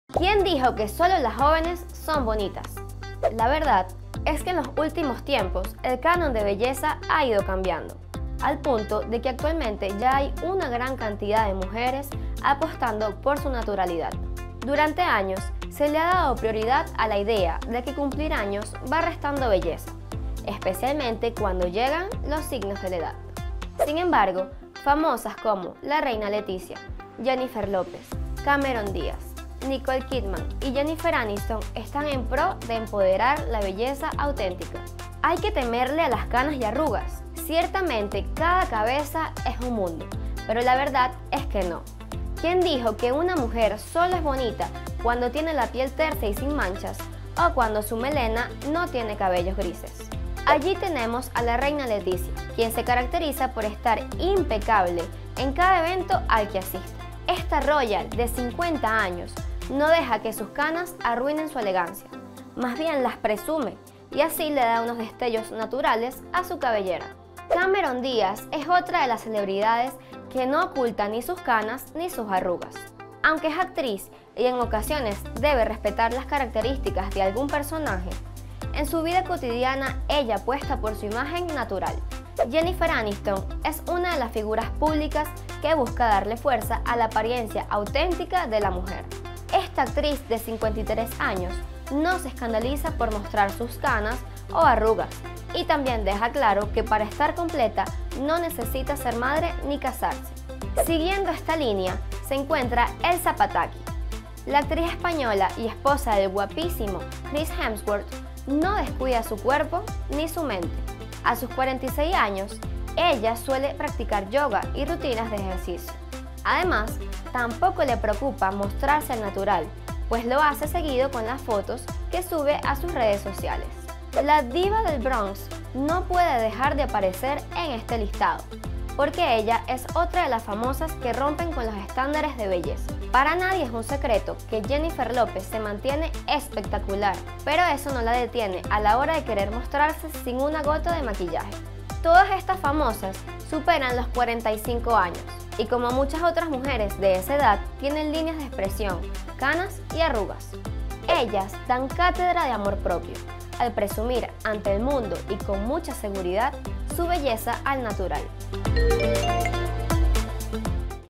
Desde Jennifer López hasta la reina Letizia: la poderosa belleza más allá de los 45
La edad no marca un límite para dejar de seguir luciendo bella y fabulosas y estas famosas lo demuestran.